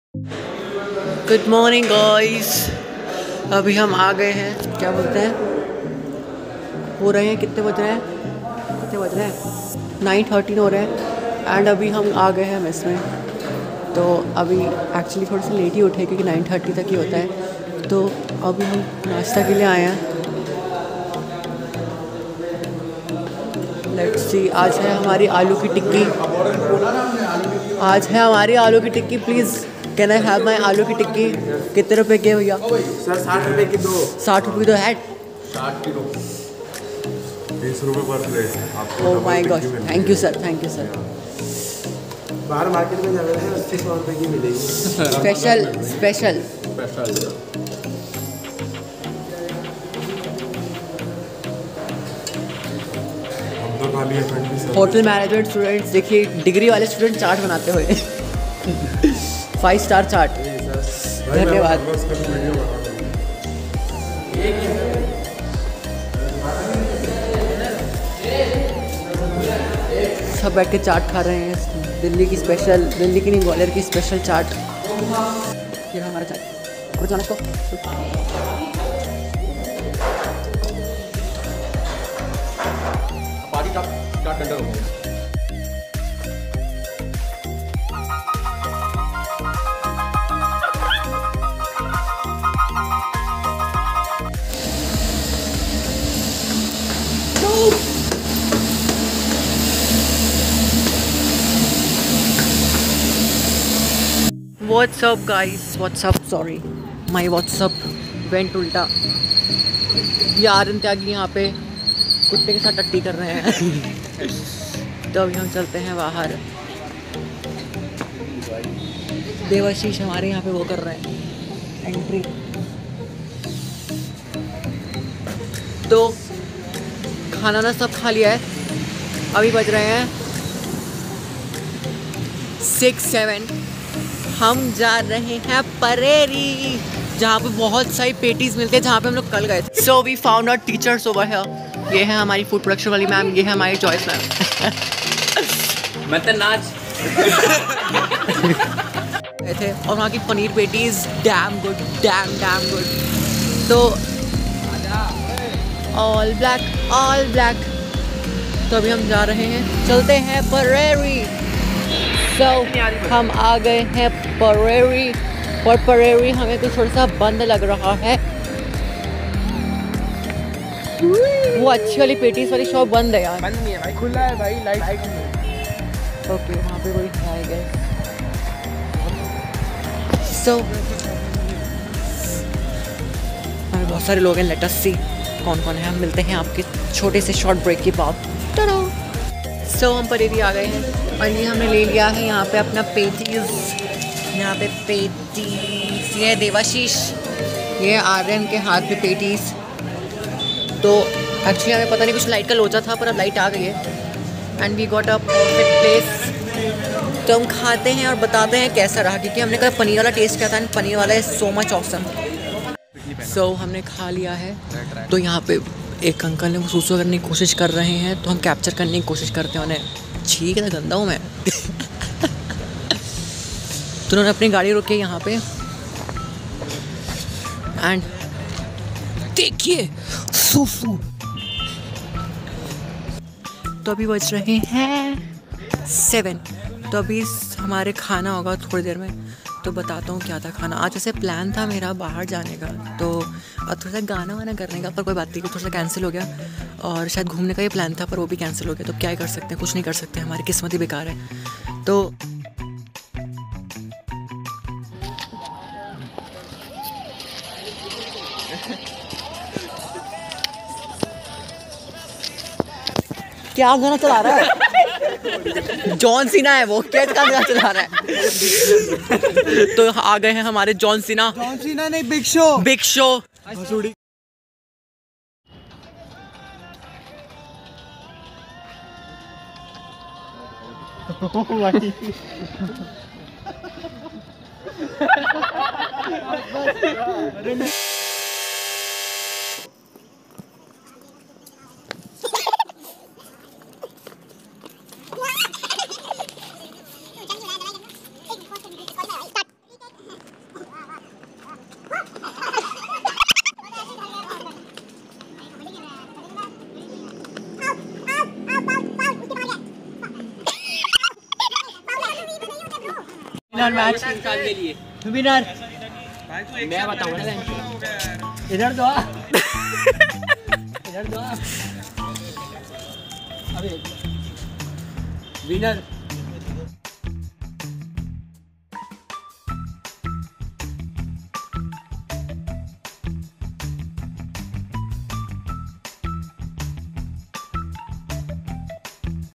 The war of LOC || College/Hostel Vlog || Hotel Management || Manthan Mishra
Hehe, I forgot to mention the meaning of LOC in the video.
Actually It is a imaginary border which separates boys hostel and girls hostel.
Thanx for watching.